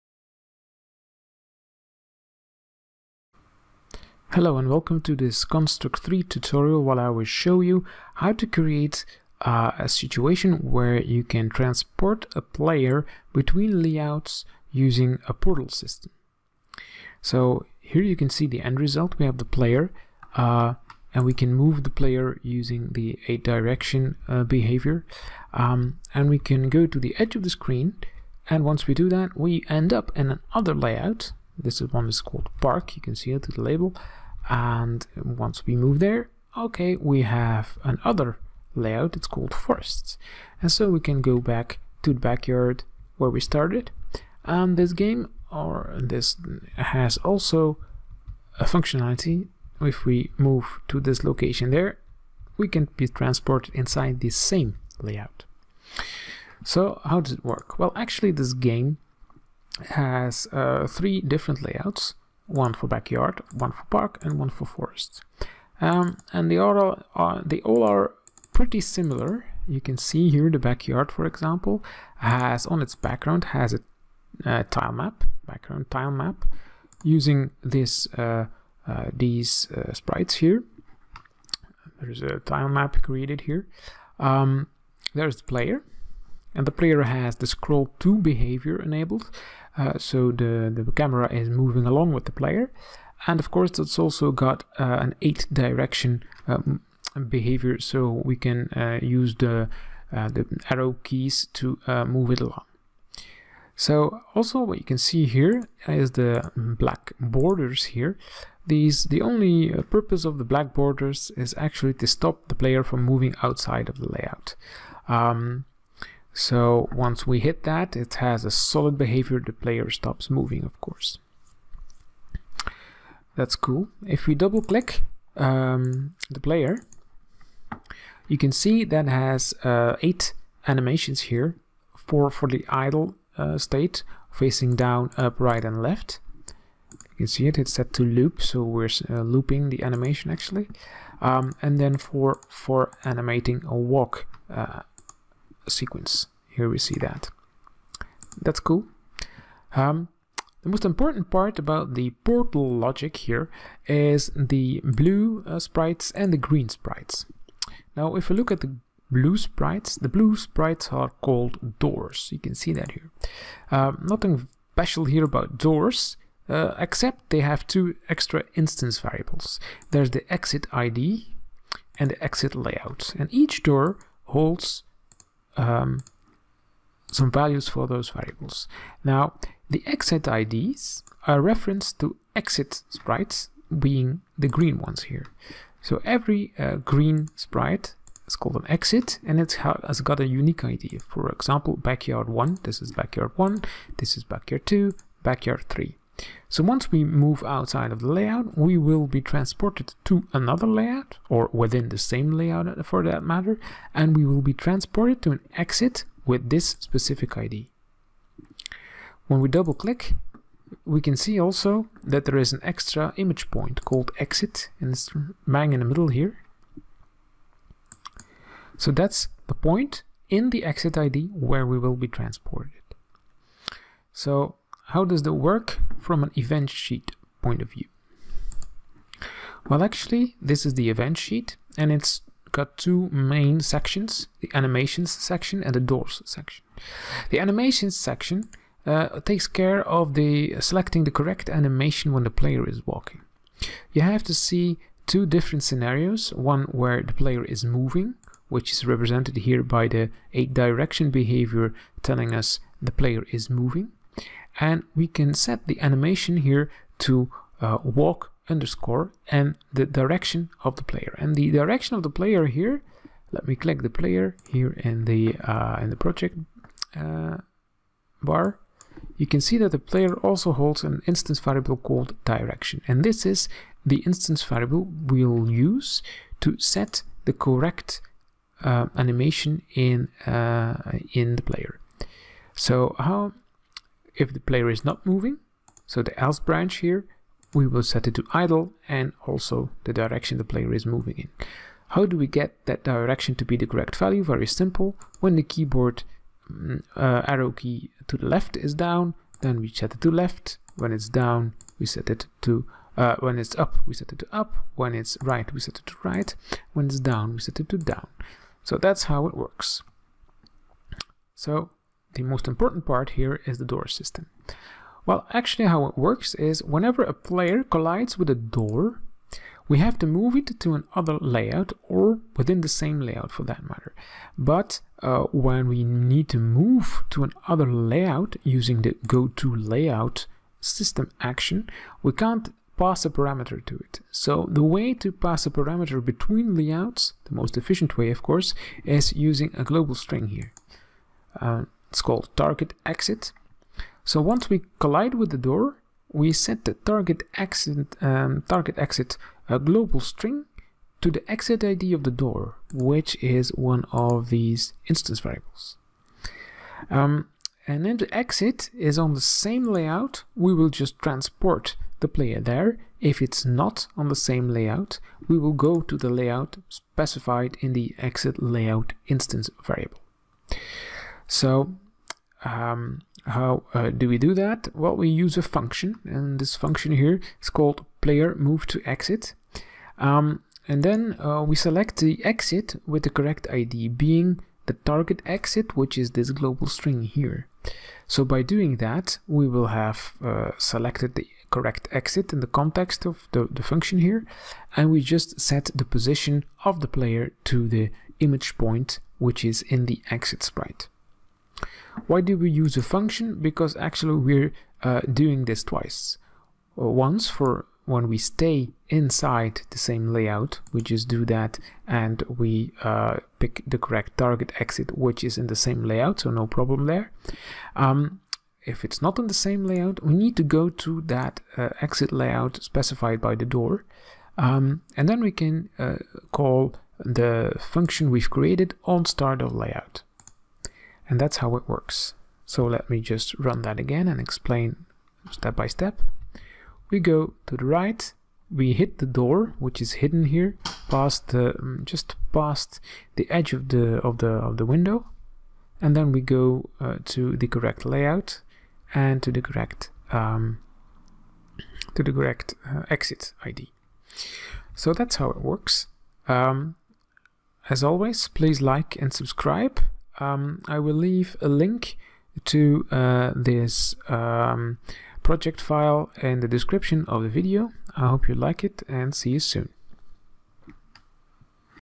Construct 3 Demo's : portal between layouts
This Construct 3 tutorial shows you how to implement portals between layouts.    You can get the full template for free